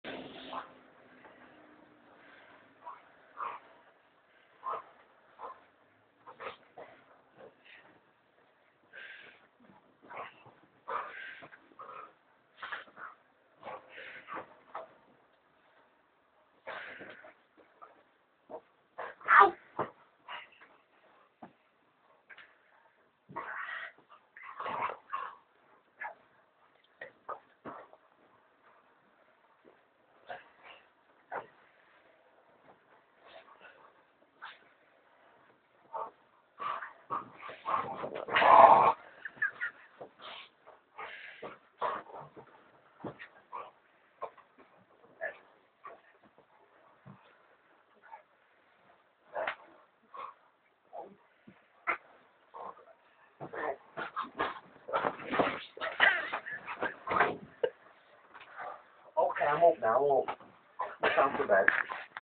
alarm call
our amstaff does not like us laying in bed